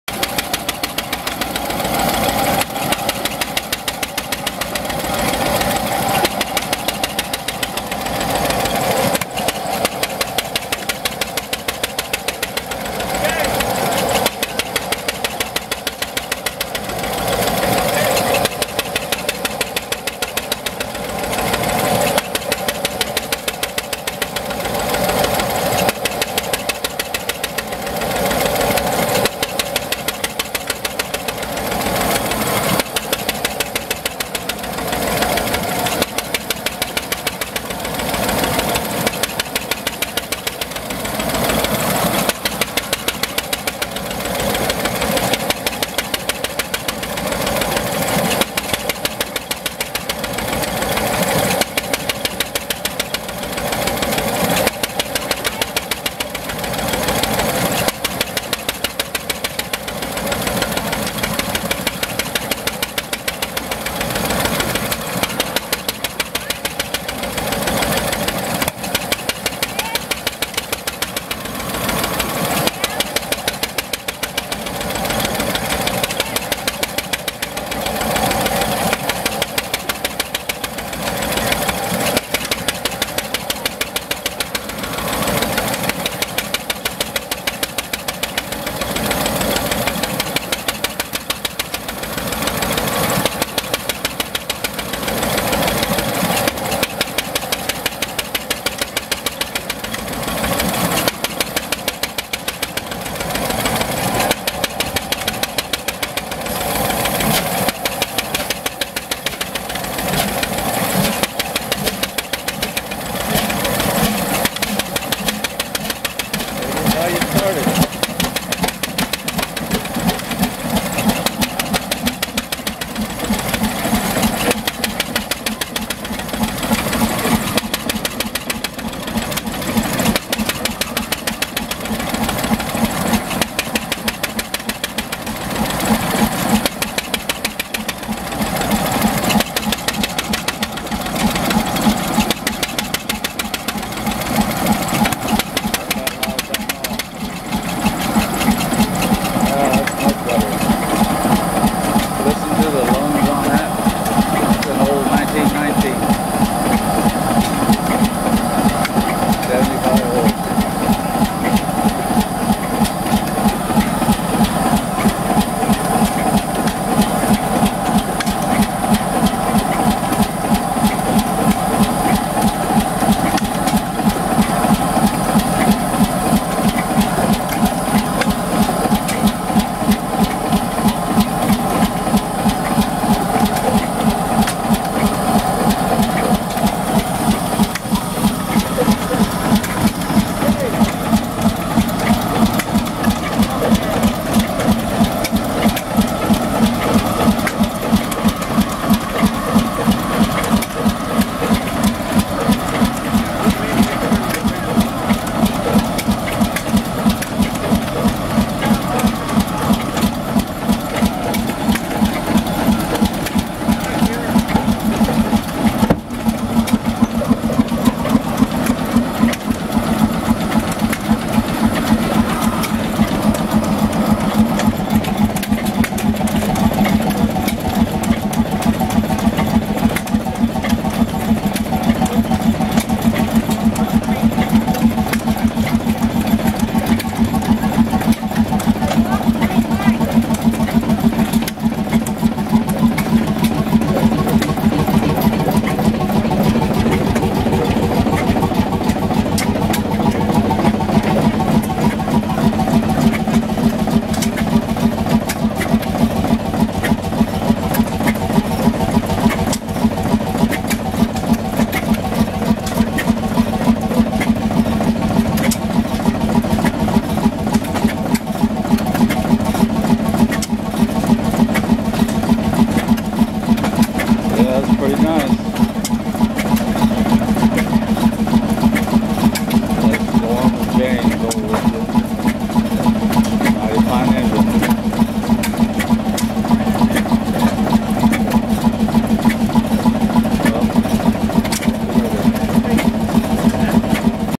1919 75 HP Fairbanks
Norm and Jane start up their old 2 cylinder diesel engine. Quite a sight. They are a good team!